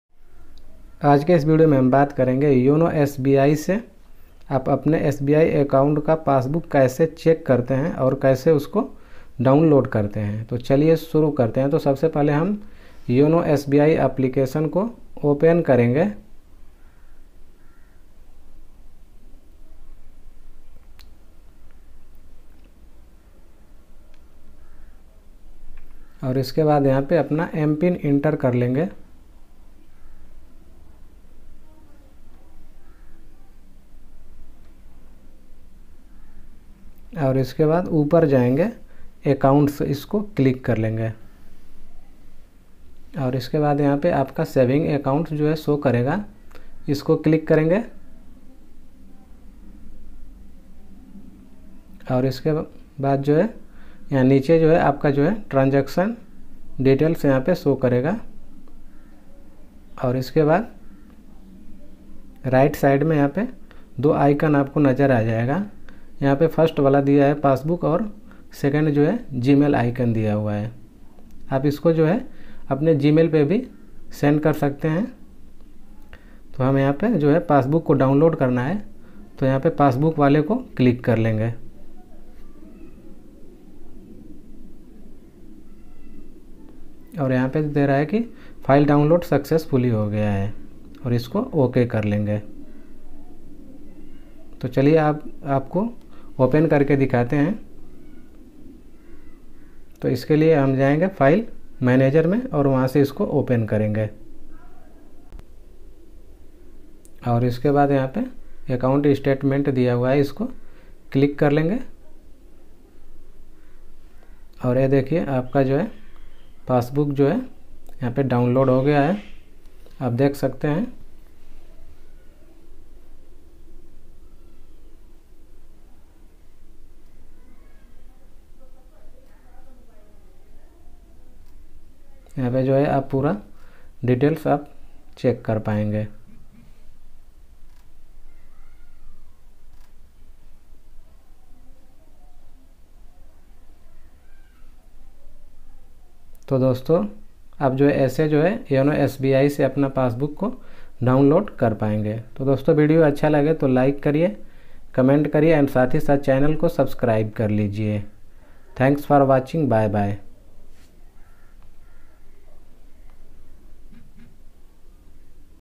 Yono sbi download passbook | How to download passbook from yono sbi | view sbi statment
Hi Anil techno ki me aap ka bahut bahut swagat hai.

About video:-
     es video me aap ko batata gya hai ki aap yoni sbi ki madat se aap ghar bathe apna passbook download kar sakte hai.  ..and view statment.